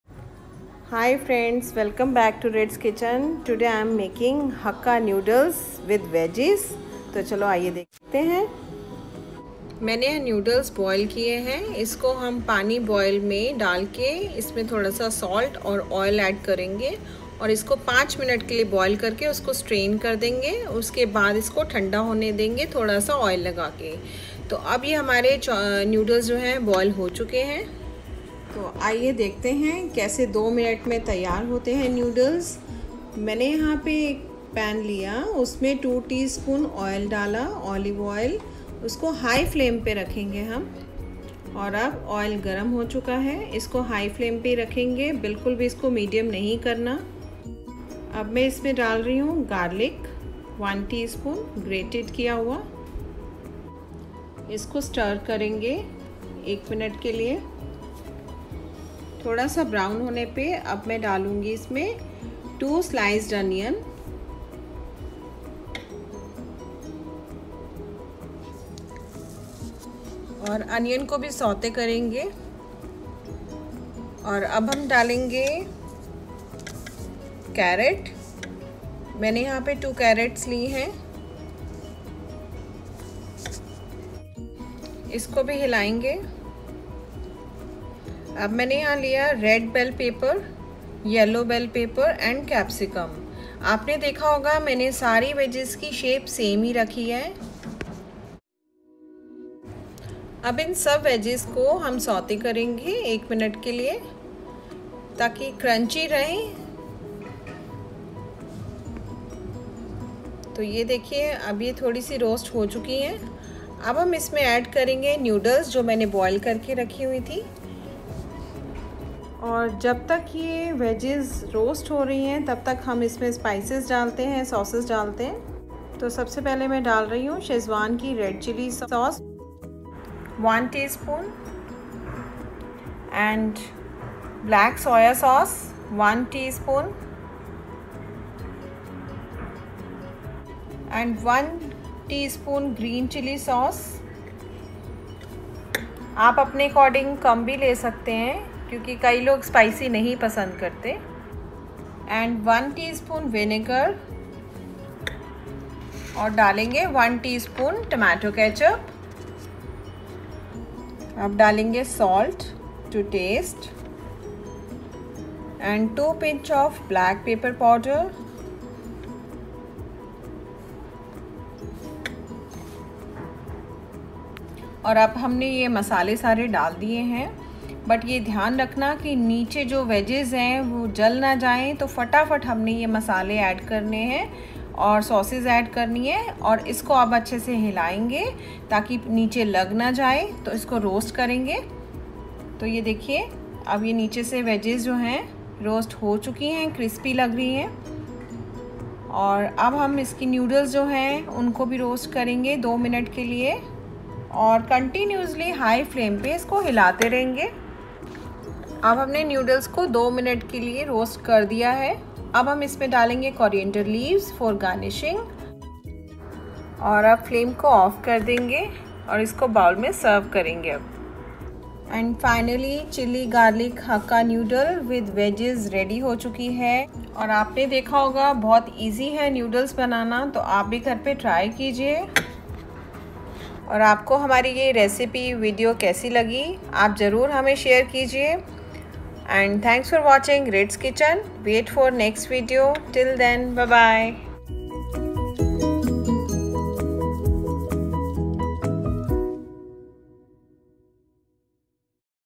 Chilli Garlic Hakka Noodles/Veg Noodles | Home Made Quick Recipe || The Ritz kitchen
Ingredients:-
Chilli garlic hakka noodles with veggies 🌶
garlic paste 1 tsp grated
Sliced onions 2
Carrot 🥕 2
Red bell pepper 1/2 bowl
Yellow pepper 1/2 bowl
Capsicum 1/2 bowl
Noodles boiled 2 bowl
Spices :-
1. Red chilli sauce 1 tsp
2. Black soya sauce 1 tsp
3. Green chilli sauce 1 tsp
4. Vinegar 1 TSP
5. Tomato ketchup 1TSP
6. Salt to taste
7. Black pepper 2 pinch of
8. Chopped coriander leaves